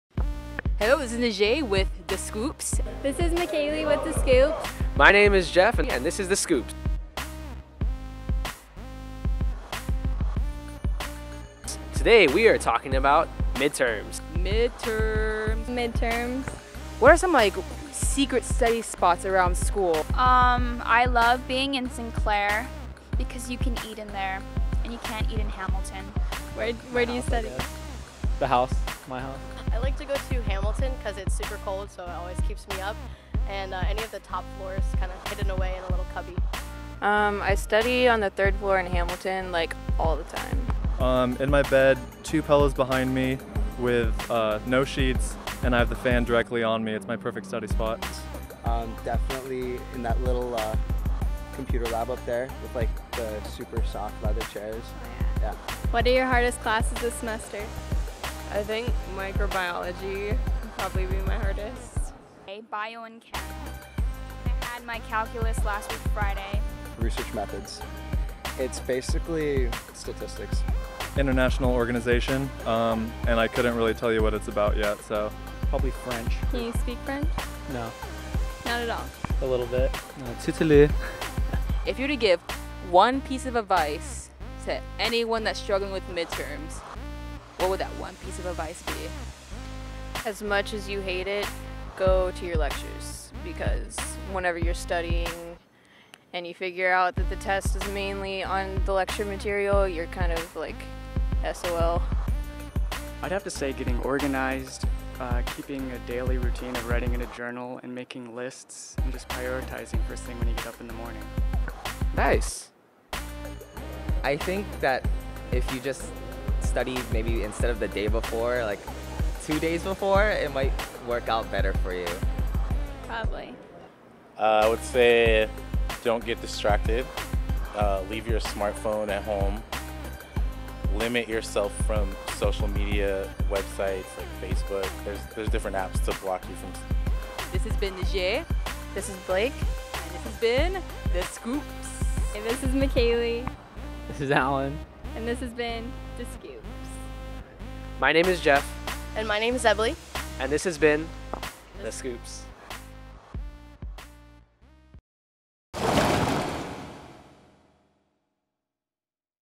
Da Scoops - Midterms
We present another exciting installment of UHP's Da Scoops!
This week we are talking about Midterms!